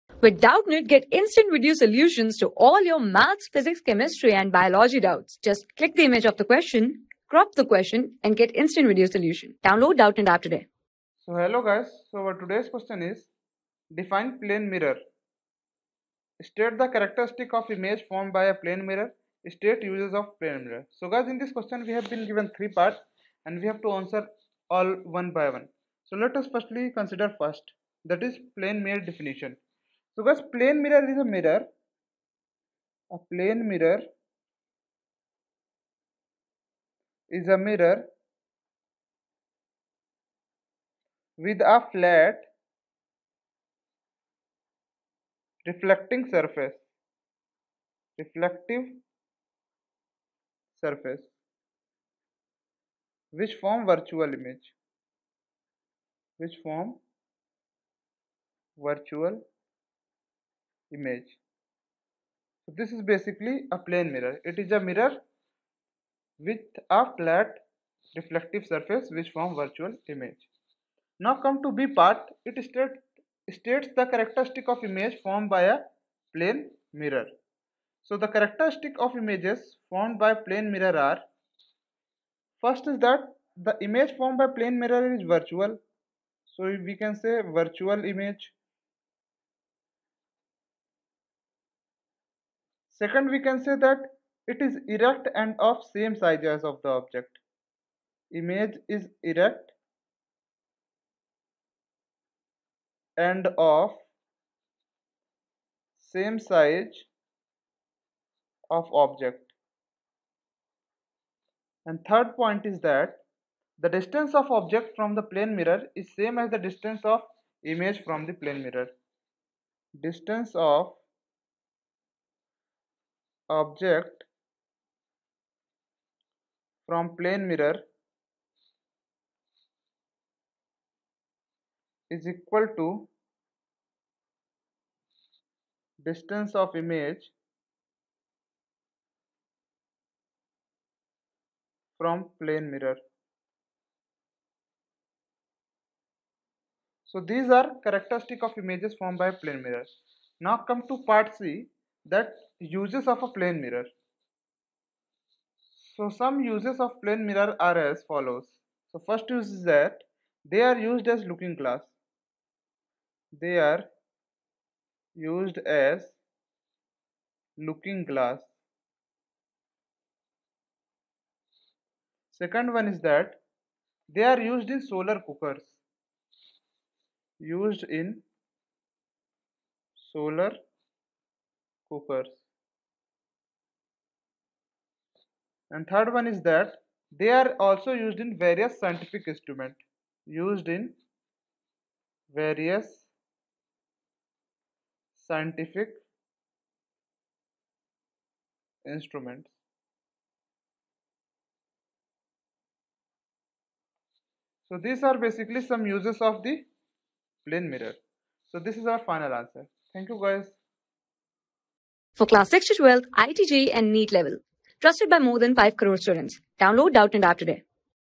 (a) Define plane mirror. (b) State the characteristics of image formed by a plane mirror. ( c ) ...
(a) Define plane mirror. (b) State the characteristics of image formed by a plane mirror. ( c ) State uses of plane mirroe.
Class: 8
Subject: PHYSICS
Chapter: LIGHT ENERGY
Board:ICSE

👉 Have Any Query? Ask Us.